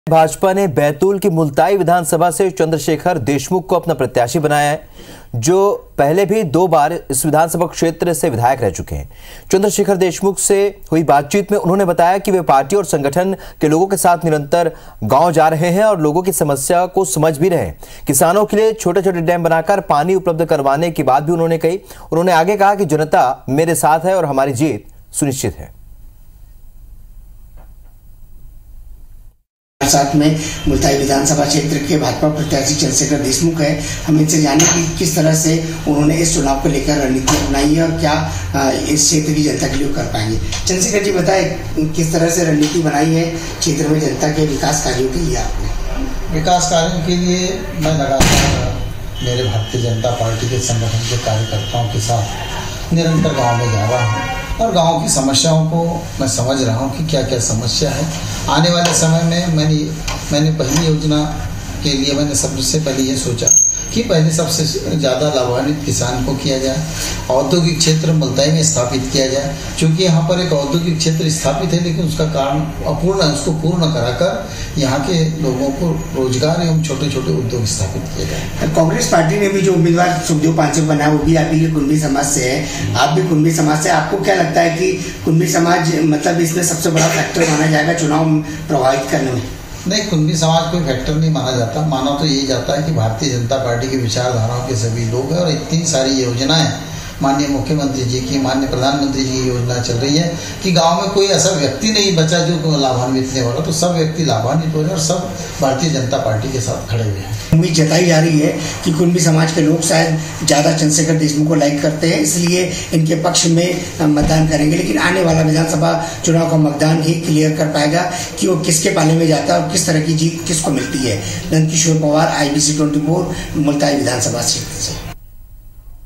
Multai Vidhansabha से BJP ने Chandrashekhar Deshmukh को दिया टिकट। BJP प्रत्याशी से खास बातचीत।देखिए
IBC24,Multai Vidhansabha से BJP ने Chandrashekhar Deshmukh को दिया टिकट। BJP प्रत्याशी से खास बातचीत।देखिए